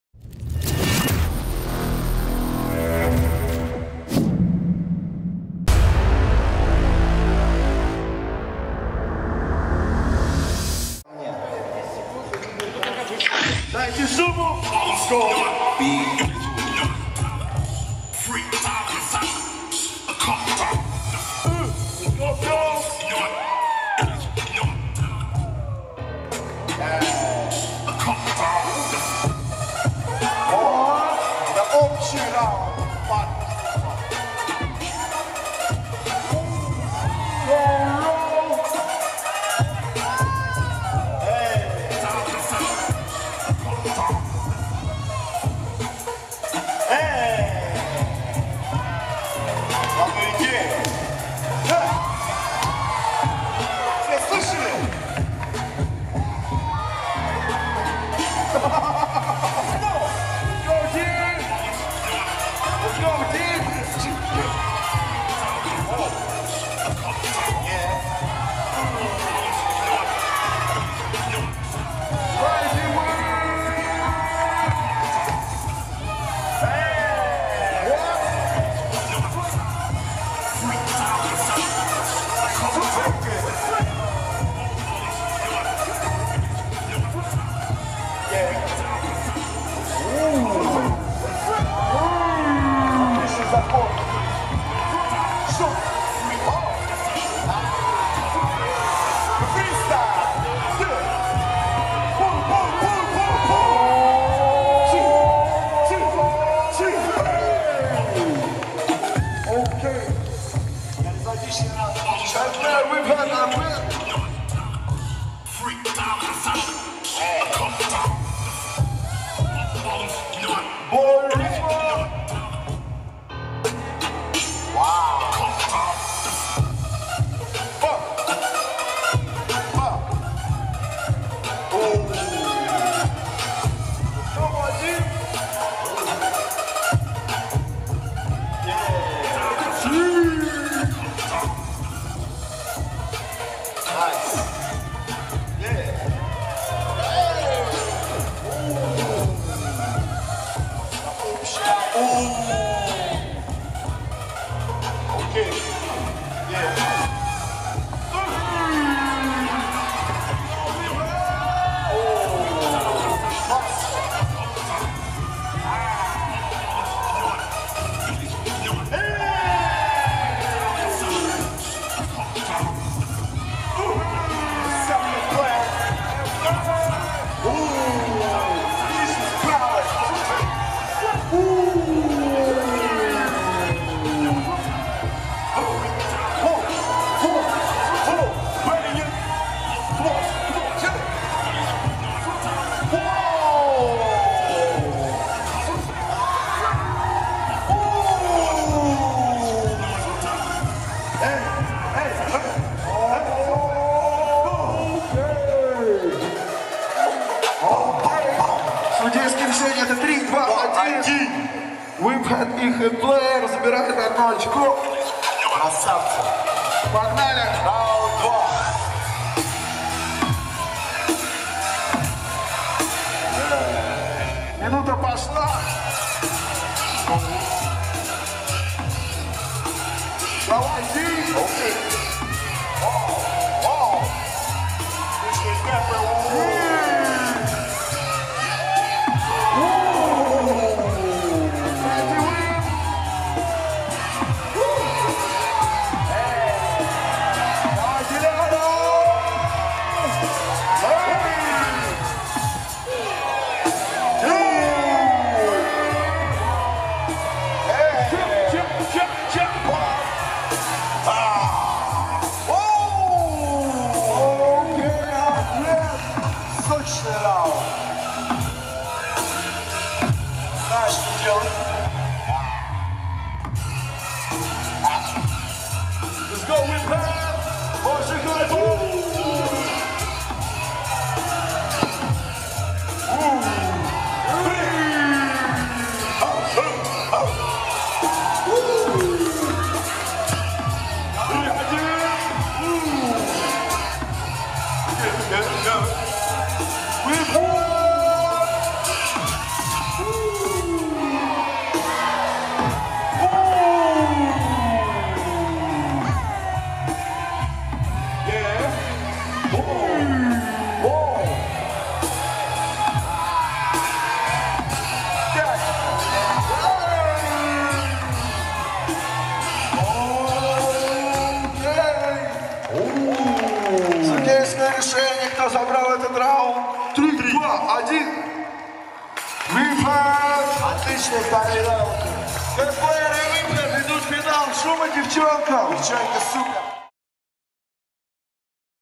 Girl & Lady Whiphead vs Whiphead & Hatplayer | PARTNERS IN BUCK 1/4 FINAL | THE KRUMPIRE 2020
6TH edition of THE KRUMPIRE RUSSIA. 21-30 July Anapa, Russia. The last time at the legendary place in Anapa with best krumpers of CIS! Workshops, events, sessions, parties and even more!

Video by SMMZ Production